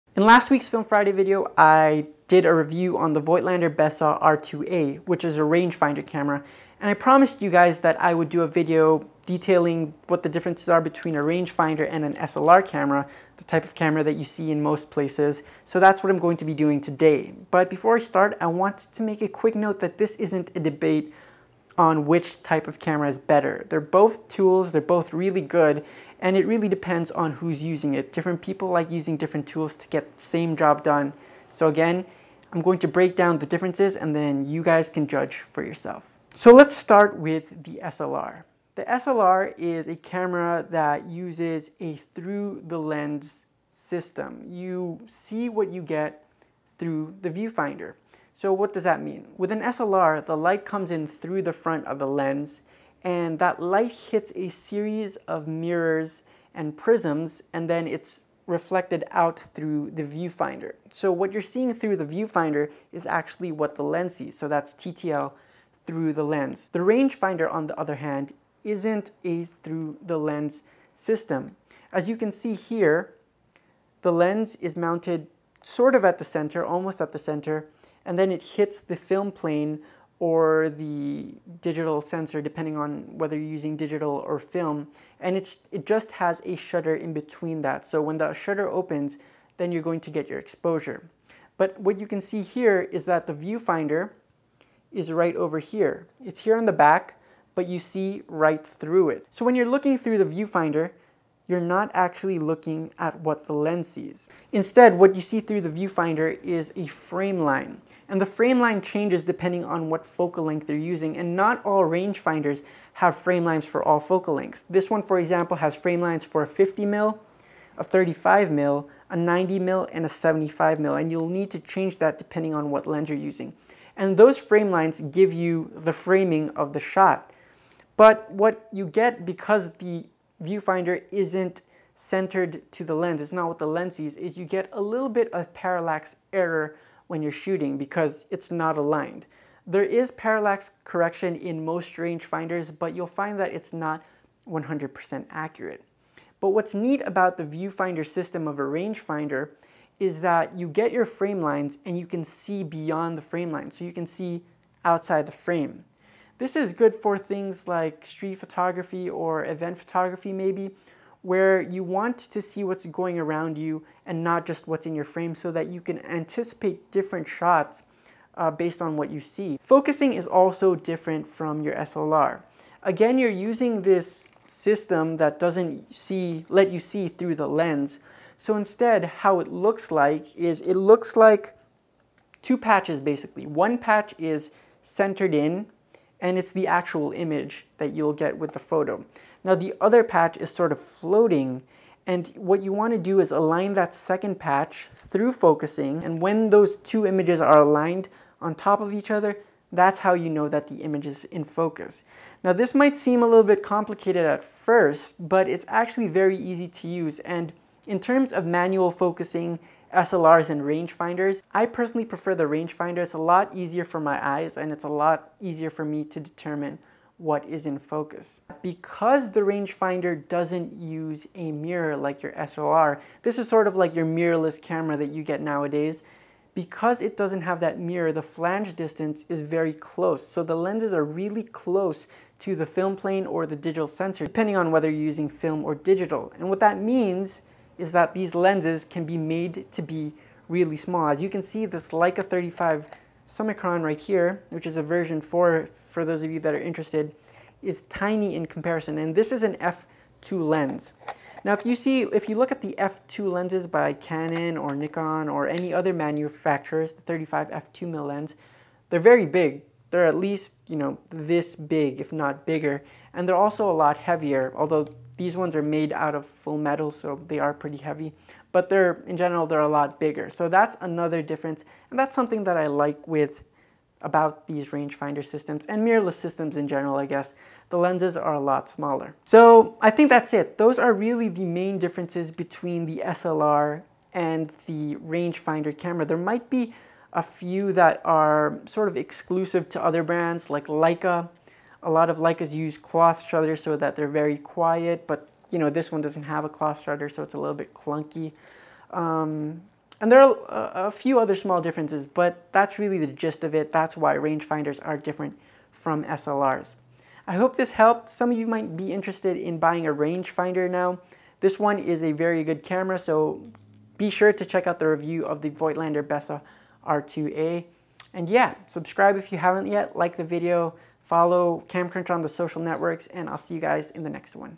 Rangefinder vs SLR Camera
What are the differences between Rangefinders and SLR Cameras? Watch to find out!

Try Amazon Prime for free and help support us

My Equipment